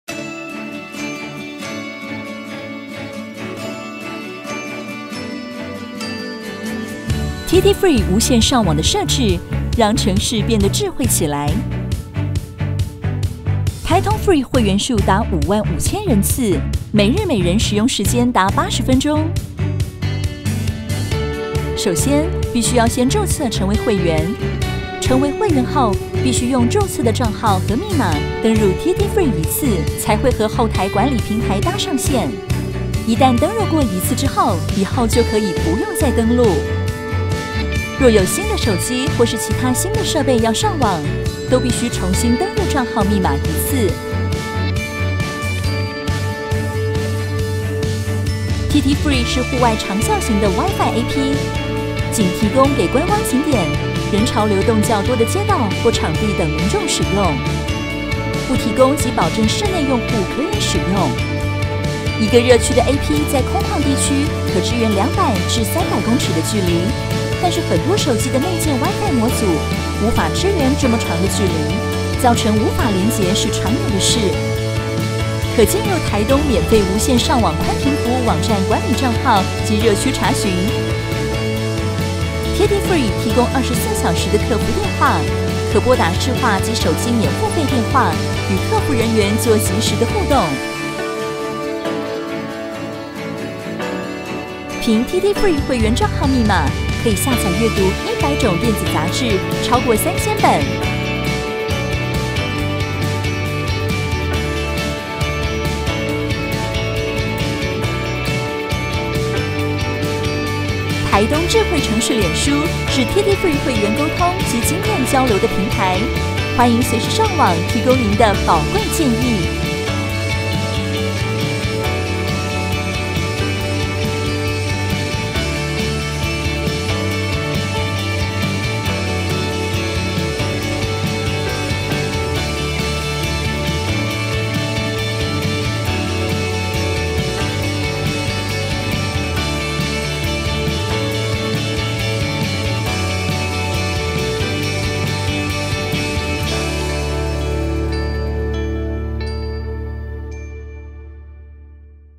TT Free 使用方法影片
臺東縣(台東縣) 無線寬頻 Taitung Free ( TT-Free ) 操作使用說明影片。